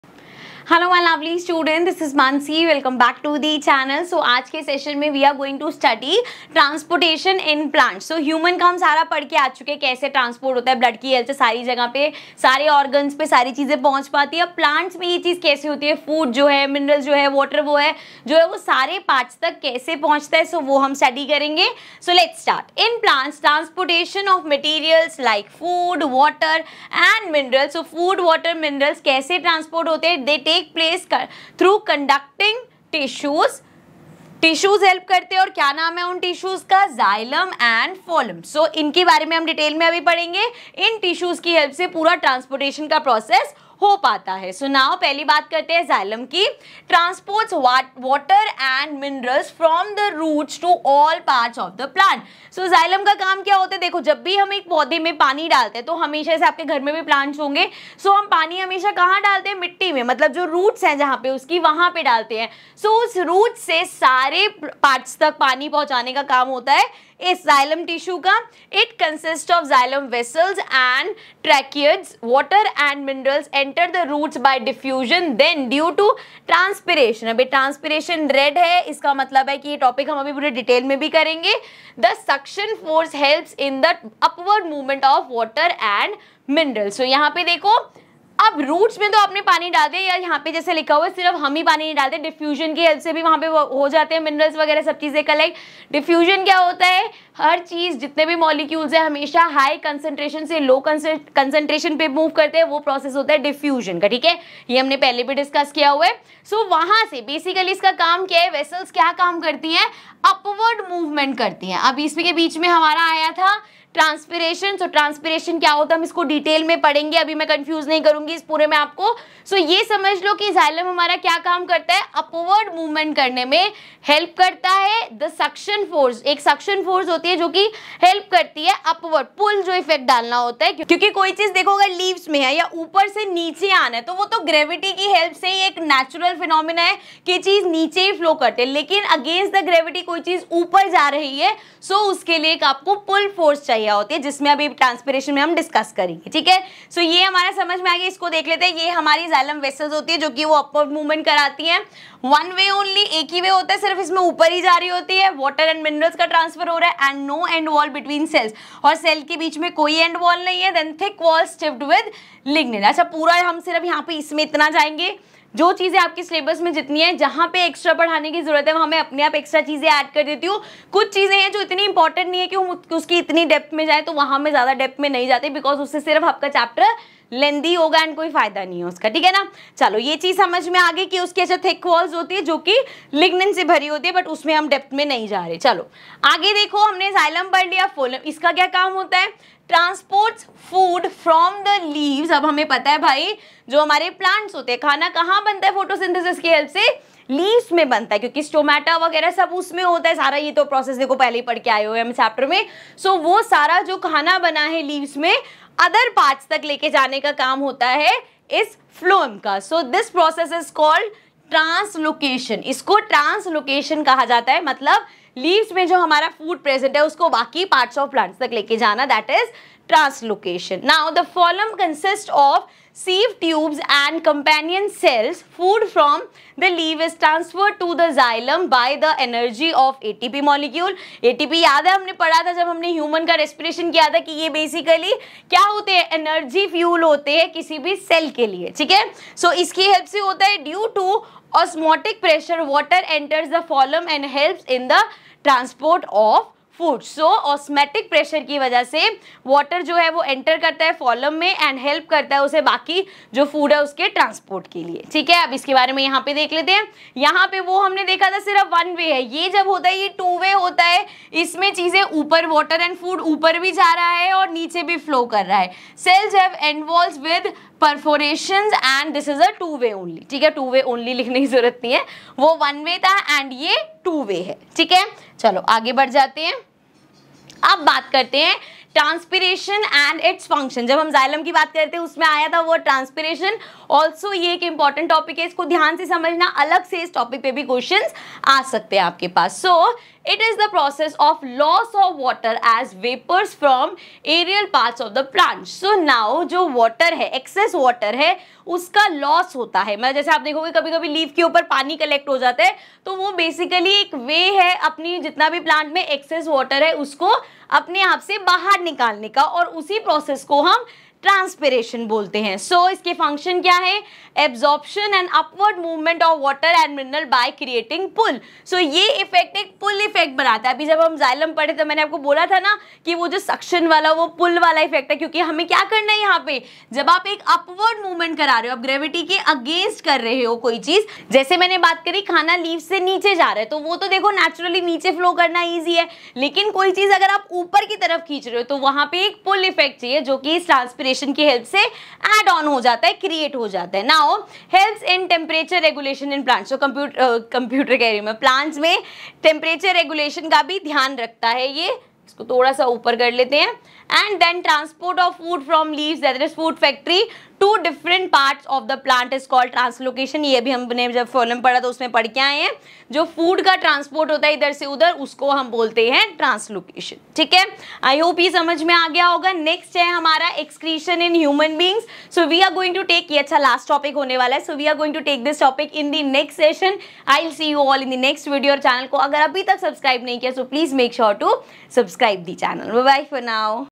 Transportation In Plants | Chapter 5 | Life Processes | Class 10 Science | NCERT
In this session, I will continue with the Life Processes chapter of class 10.